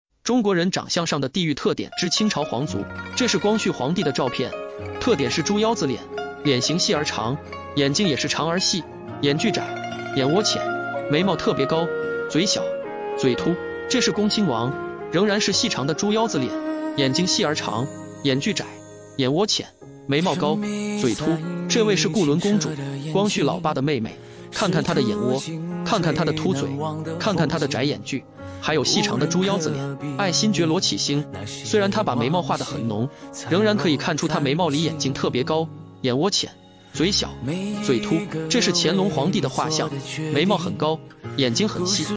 中国人长相上之清朝皇族篇。中国人|长相|清朝|皇族|爱新觉罗|乾隆|光绪|恭亲王|固伦公主
中国人长相上之清朝皇族篇，清朝皇族的长相有什么特点呢